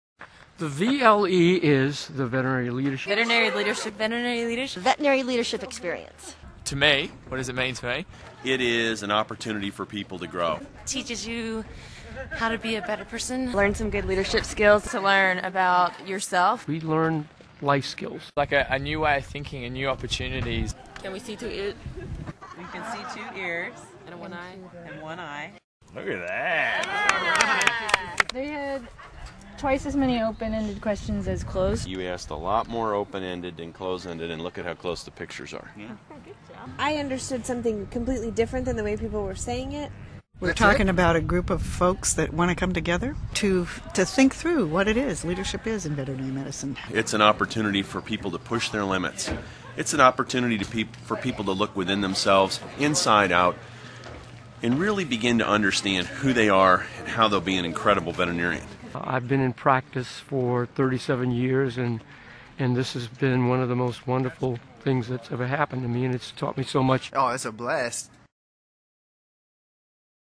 AVMA Veterinary Leadership Experience
Hear from those who attended this year's Veterinary Leadership Experience, and how many believe it has changed their lives. Powered by WSU and its Animal Industry Partners